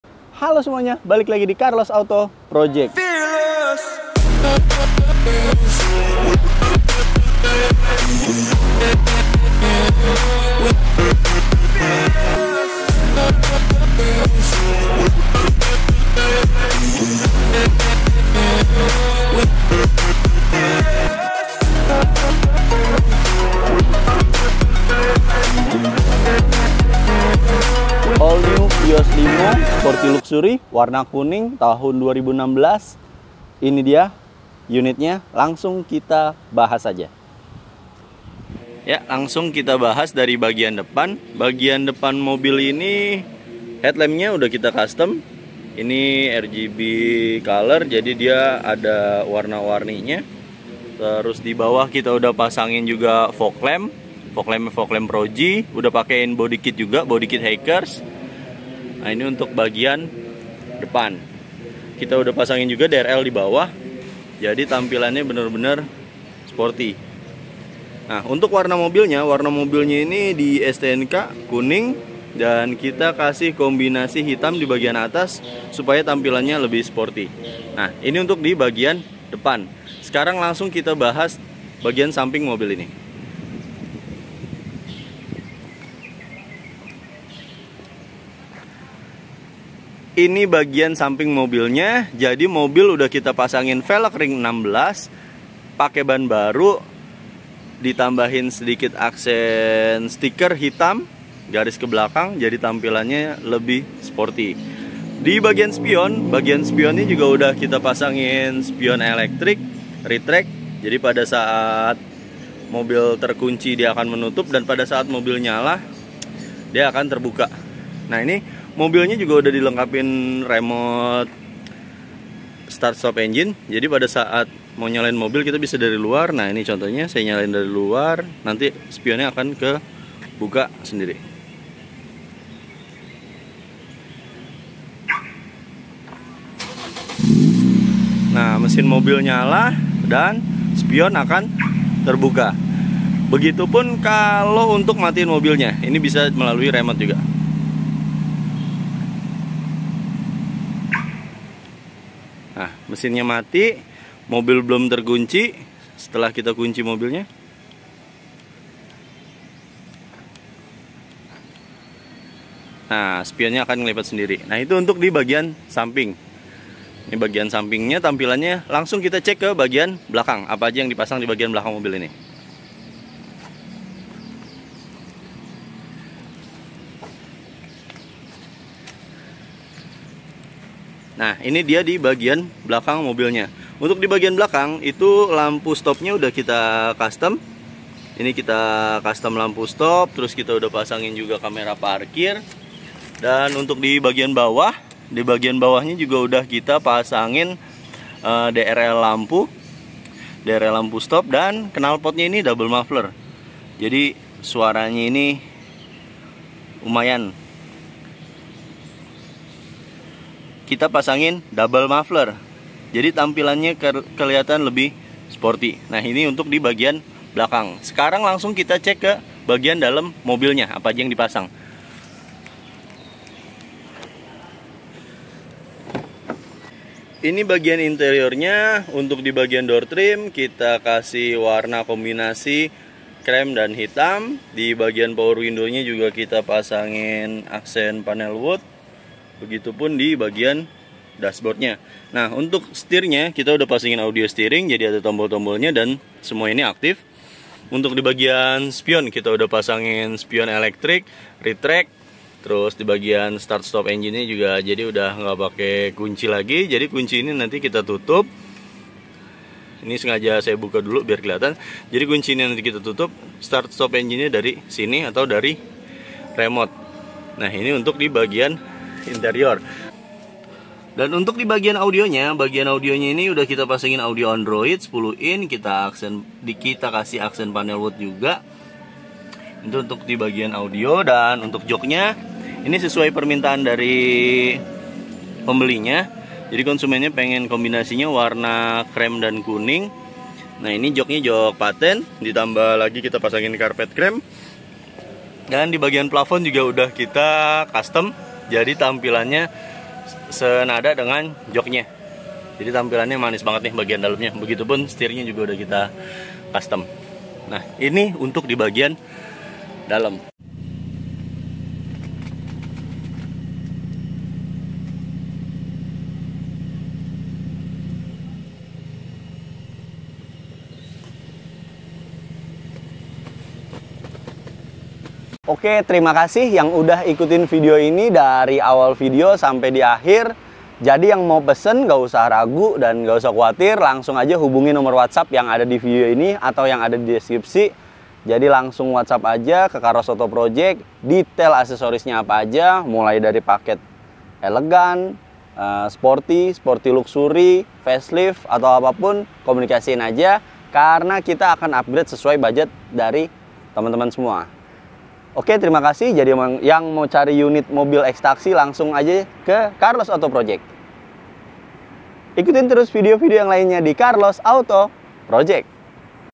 Modifikasi Ternyaman | Vios Limo Sporty Luxury | Carlos Build
Mobil ini adalah Vios Limo eks Taksi Blue Bird yang telah di Modifikasi dibagian Eksterior dan Interior sesuai dengan keinginan dan budget dari konsumen. semua dikerjakan secara maksimal oleh Carlos Auto Project.

Info Detail :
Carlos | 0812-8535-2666